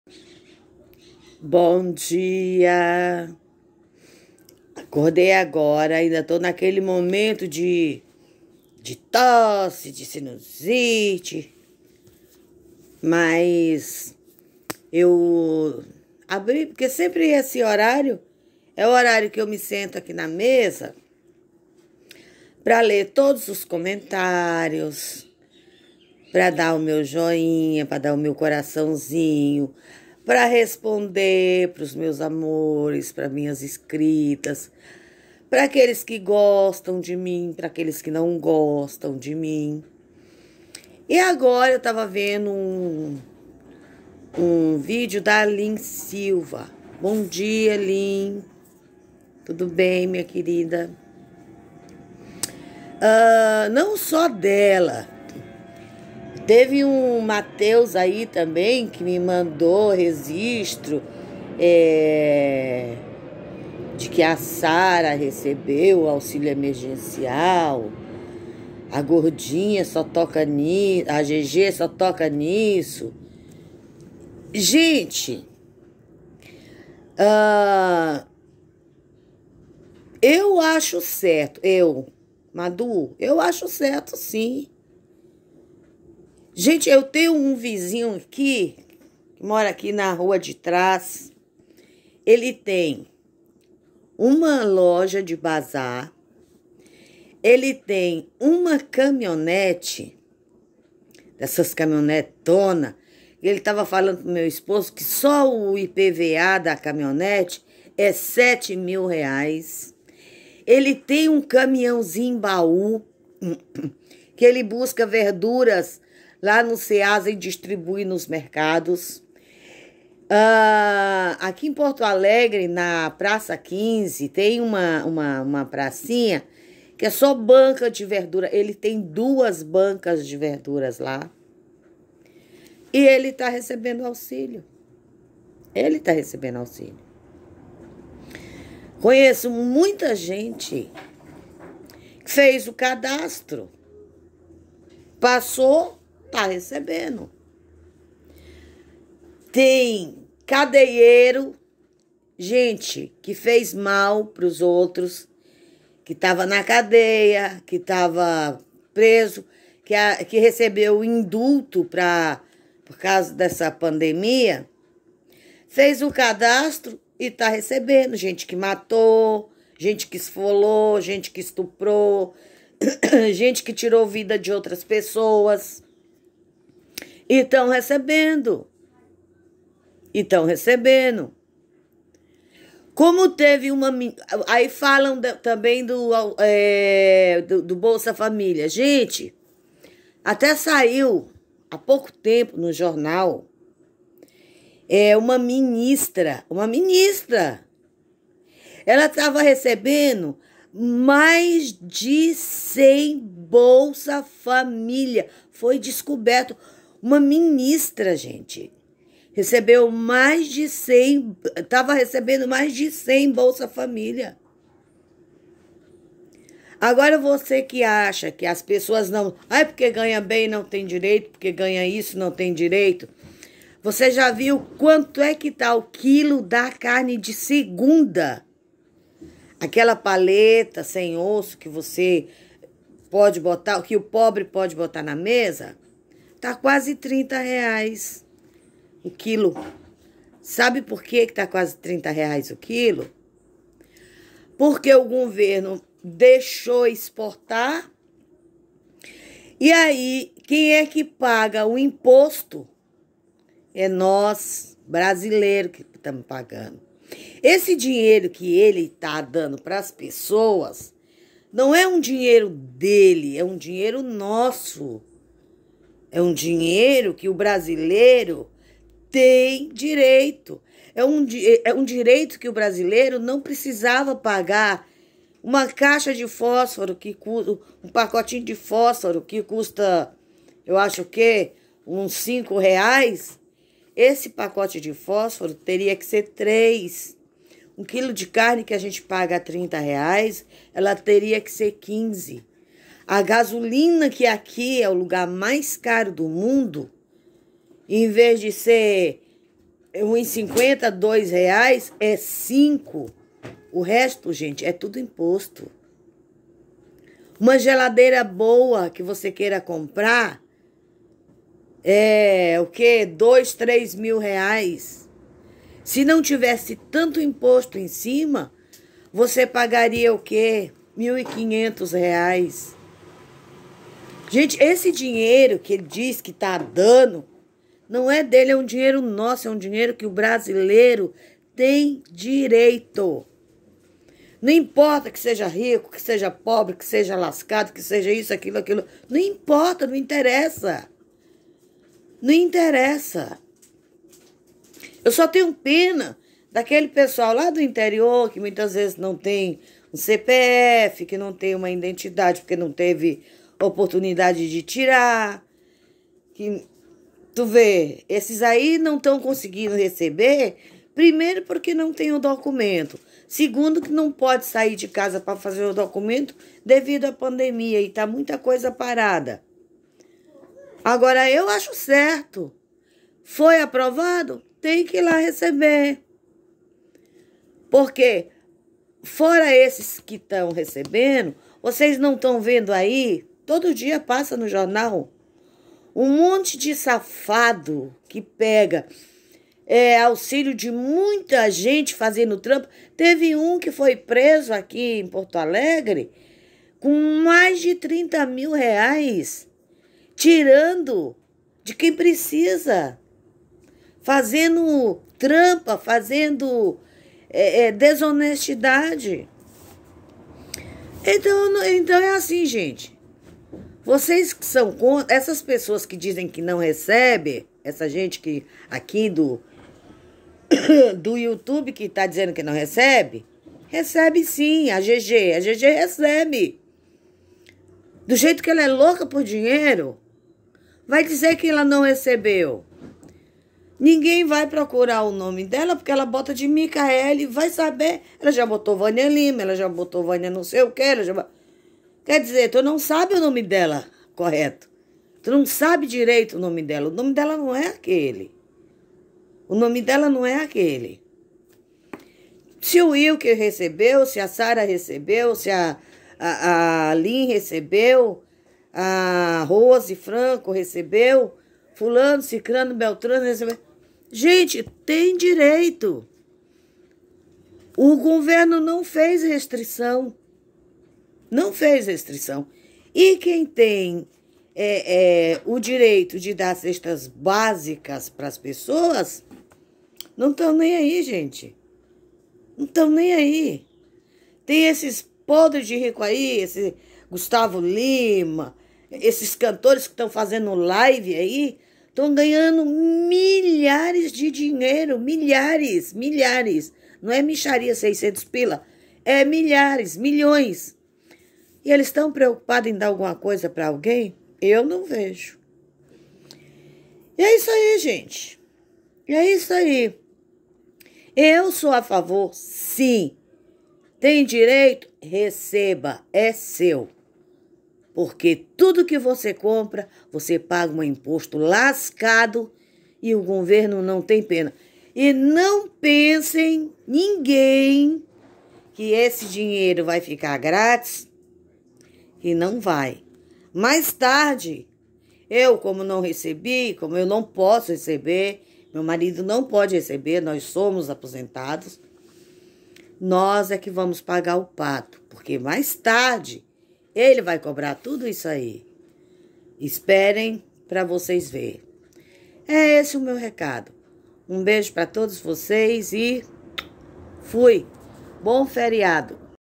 QUERO DAR MEU PALPITE SOBRE O AUXILIO EMERGENCIAL.
vc tem díreito sim, esse dinheiro ja pagamos todos os dias com impostos